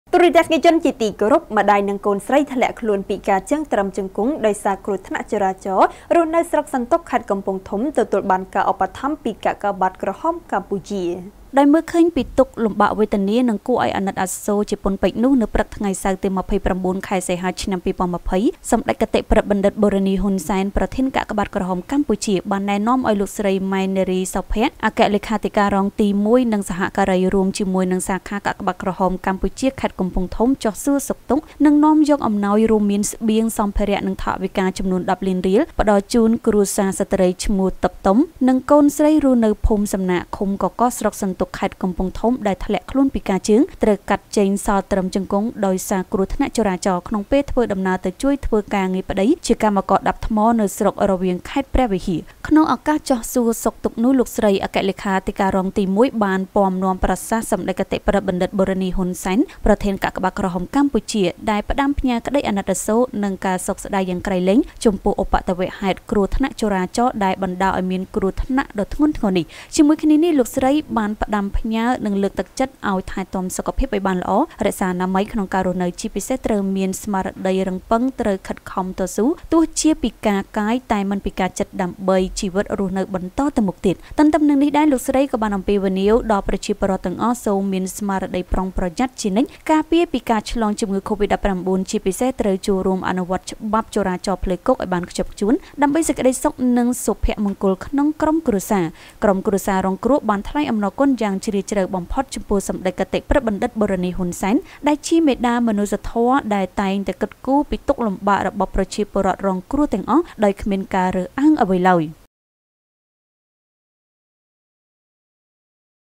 ម្តាយនិងកូនស្រីធ្លាក់ខ្លួនពិការជេីង ត្រឹមជង្គង់ ដោយគ្រោះថ្នាក់ចរាចរណ៍ រស់នៅស្រុកសន្ទុក...
ម្តាយនិងកូនស្រីធ្លាក់ខ្លួនពិការជេីង ត្រឹមជង្គង់ ដោយគ្រោះថ្នាក់ចរាចរណ៍ រស់នៅស្រុកសន្ទុក ខេត្តកំពង់ធំ ទទួលបានការជួយឧបត្ថម្ភ ពី កាកបាទក្រហមកម្ពុជា* រាយការណ៍ព័ត៌មានដោយកញ្ញា យឹម ចាន់ស្រីវីន  និងកញ្ញា សំ  ស្រីនីត ,ទូរទស្សន៍ Fresh News